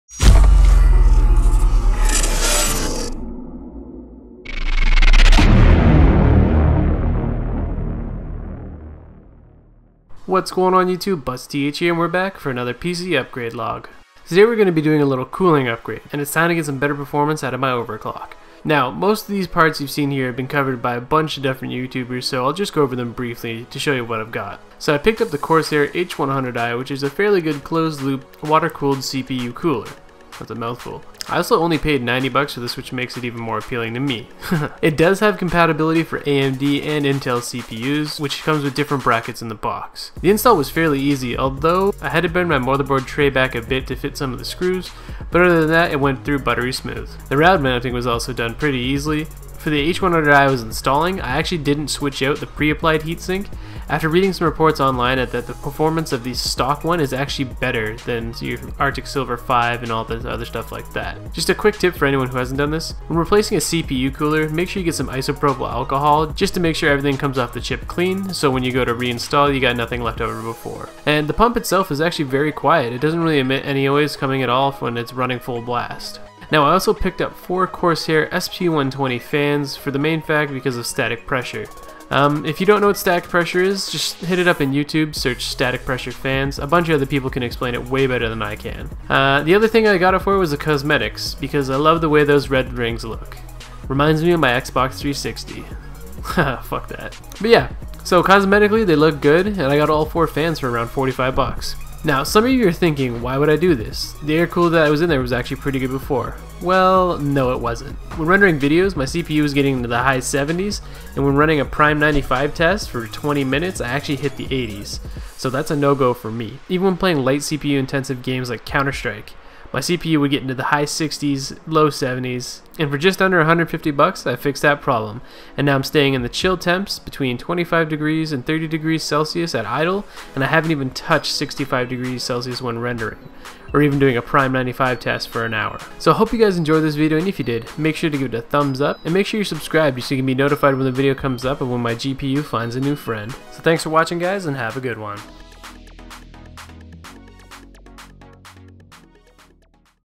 Custom PC Cooling Upgrade! - H100i + SP120 Fans - The Rig EP2 - Upgrade Log
back with #2 of the PC upgrade log, this time we add some good cooling to my overclocked i53570k and bring those temps down very nicely.